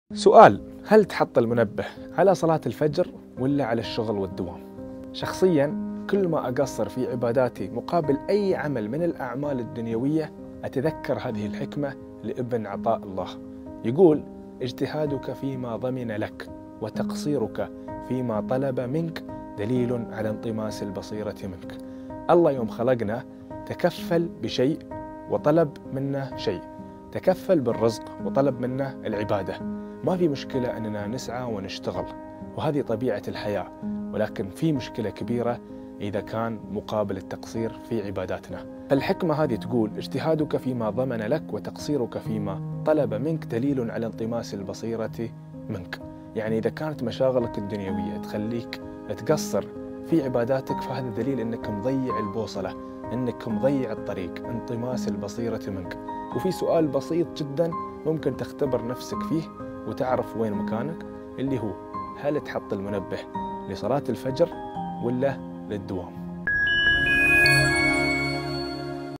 العبادة__الفرق بين الخشية من الله و انطماس البصيرة♥️
العبادة غاية العباد التي خلقوا لها. فاختبر عبادتك
قال رسول الله صلى الله عليه وسلم: «لن يلج النار أحد صلى قبل طلوع الشمس، وقبل غروبها»[9] ويقول صلى الله عليه وسلم: «من صلى البردين دخل الجنة»
و قوله صلى الله عليه وسلم: «ركعتا الفجر خير من الدنيا وما فيها ”.